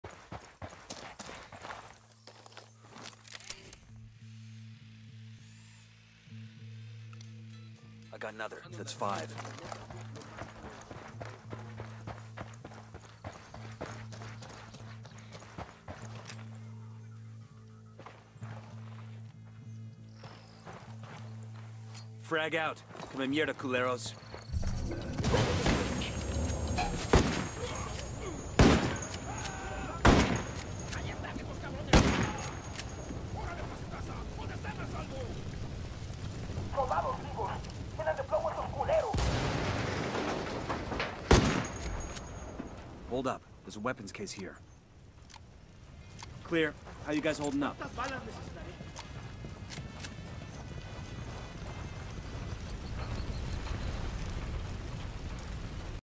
Tex Calhoun Strikes
World-renowned gunslinger, Tex Calhoun wipes out a small posse of Santa Blanca rattlesnakes.